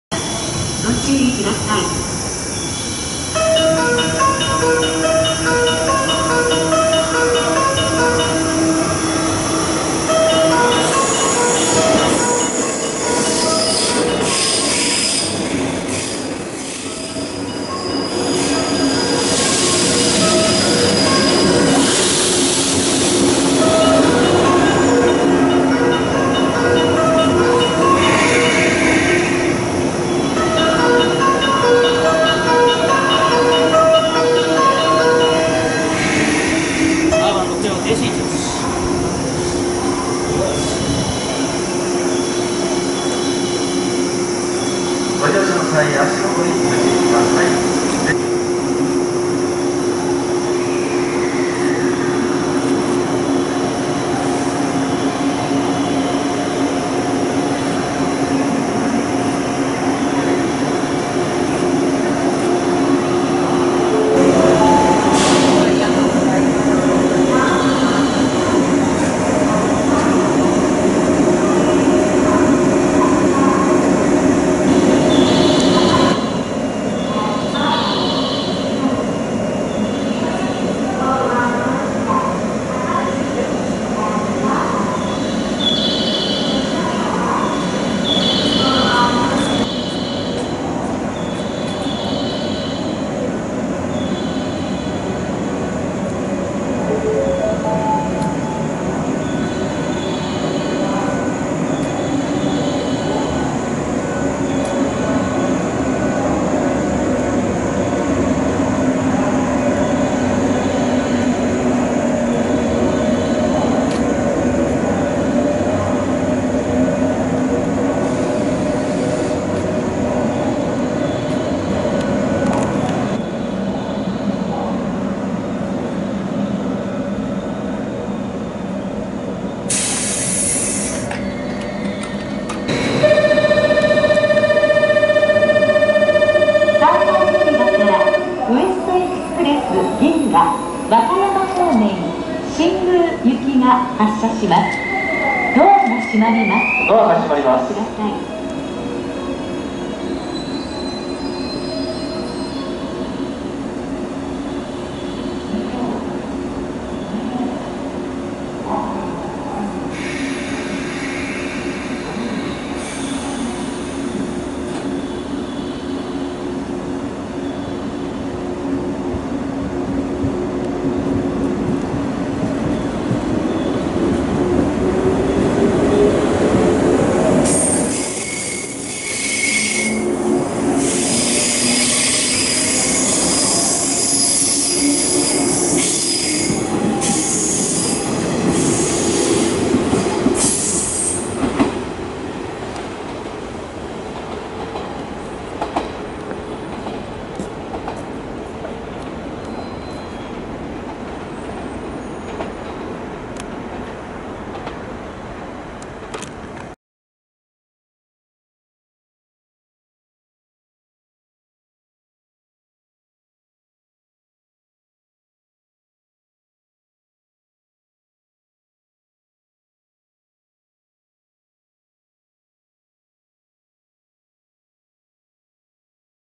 2021年11月15日ＪＲ西日本 WEST EXPRESS銀河号紀南コース 新大阪駅発着の様子を撮影してみました
2021年11月15日

ＪＲ西日本　WEST EXPRESS銀河号
紀南コース　新大阪駅発着の様子を撮影してみました

117系が見事に夜行電車に生まれ変わっていますね